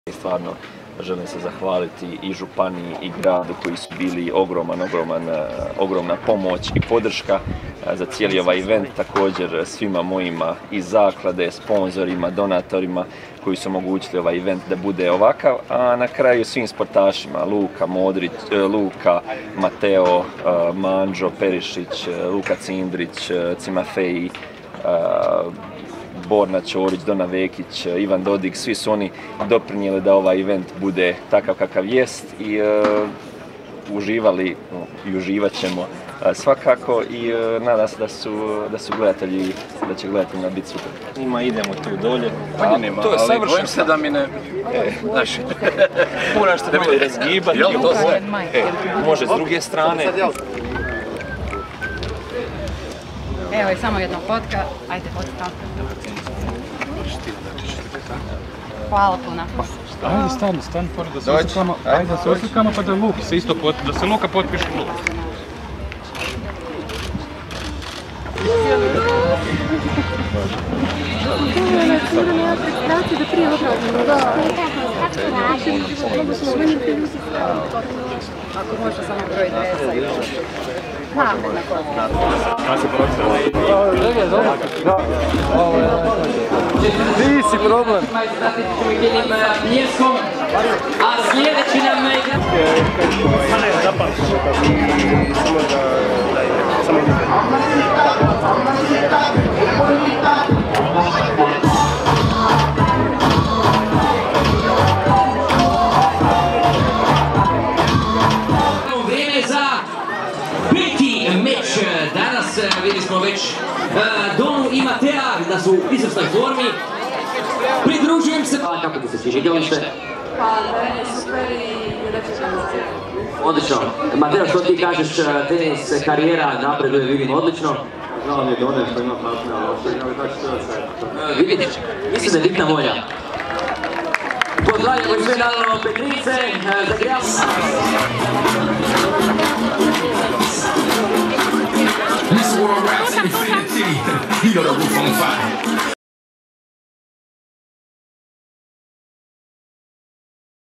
Humanitarno - sportski spektakl Gem Set Hrvatska u Petrinji
Održano je drugo izdanje humanitarno - sportskog spektakla Gem Set Hrvatska. Projekt je to koji organizira Zaklada Marin Čilić, a okuplja najpoznatije sportaše i sportašice Hrvatske s ciljem izgradnje sportskih igrališta za djecu i promocije važnosti bavljenja sportom od najranije dobi. Nastupili su Marin Čilić, Luka Modrić, Dona Vekić, Borna Čorić, Mario Mandžukić, Mateo Kovačić, Ivan Perišić, Ivan Dodig i mnogi drugi.